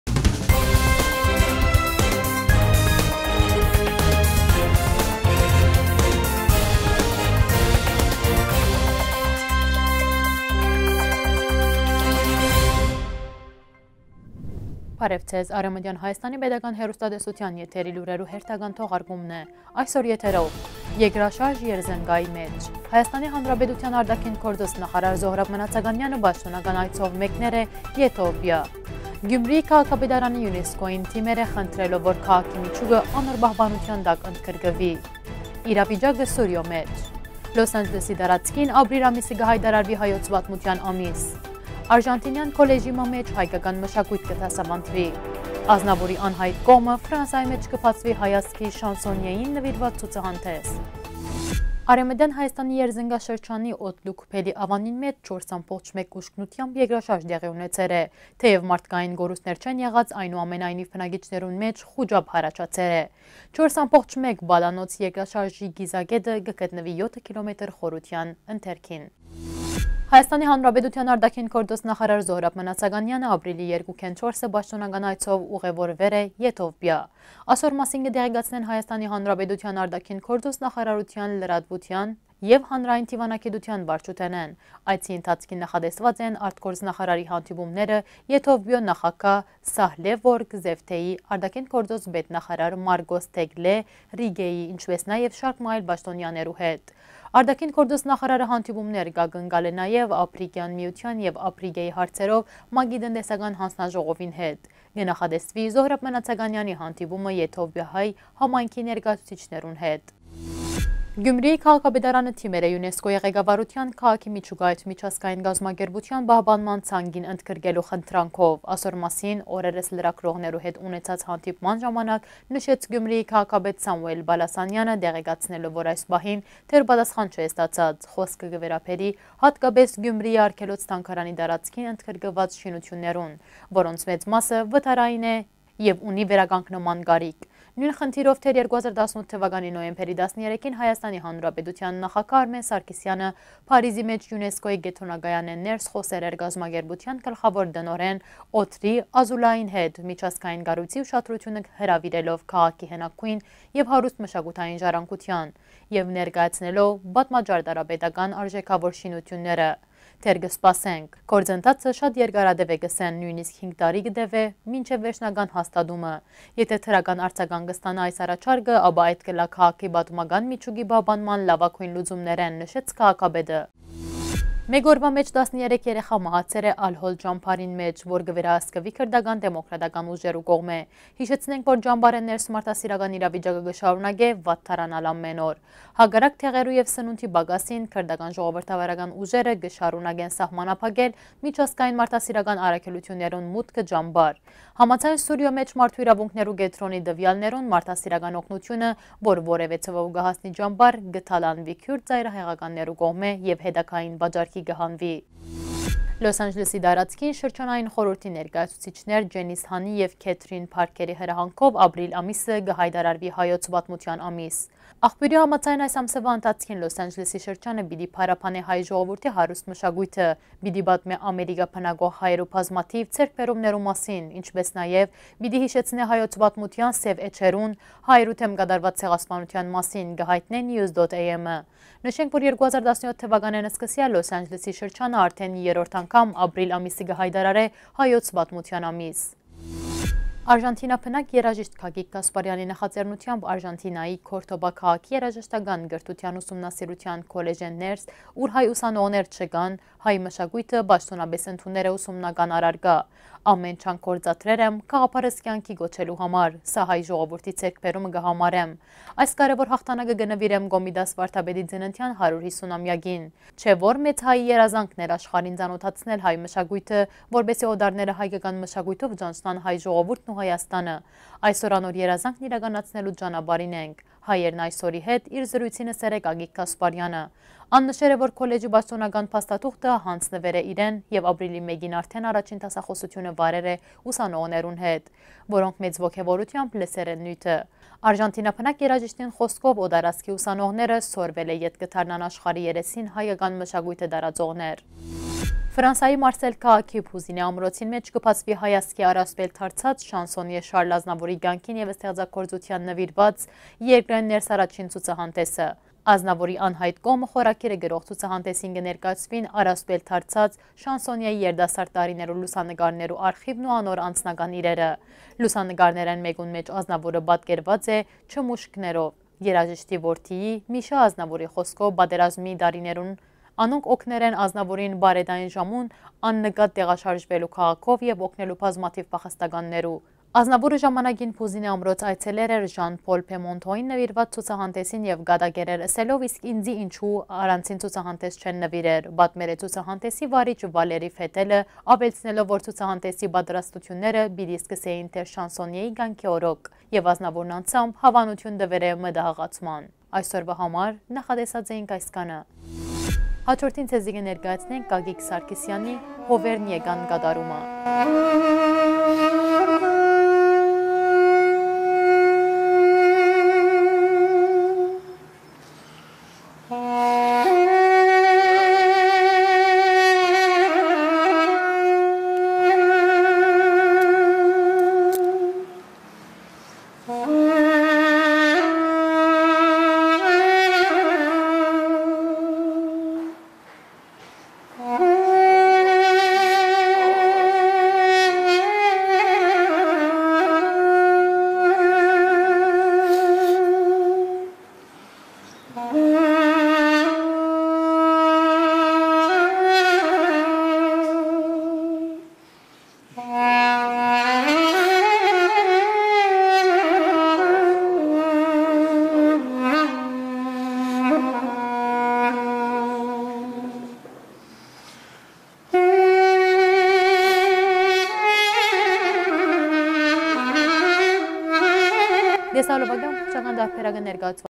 «Ազնաւուրի անյայտ կողմը». Ֆրանսայի մէջ հայազգի շանսոնիէին նուիրուած ցուցահանդէս :Լրահոս 2019-04-03
1.Երկրաշարժ Երզնկայի մէջ
2.ՀՀ Արտաքին Գործոց նախարար Զօհրաօ Մնացականեանը պաշտօնական այցով մեկներ է Եթովպիա
3.Գիւմրիի քաղաքապետարանը ՅՈՒՆԵՍԿՕ-ին դիմեր է, խնդրելով, որ քաղաքի միջուկը  անոր պահպանութեան տակ ընդգրկուի
4.Իրավիճակը Սուրիոյ մէջ
5.Լոս Անջելոսի տարածքին Ապրիլ ամիսը կը յայտարարուի «Հայոց պատմութեան ամիս»
6.Արժանտինեան քոլեջի մը մէջ հայկական մշակոյթ կը դասաւանդուի
7.«Ազնաւուրի անյայտ կողմը». Ֆրանսայի մէջ կը բացուի հայազգի շանսոնիէին նուիրուած ցուցահանդէս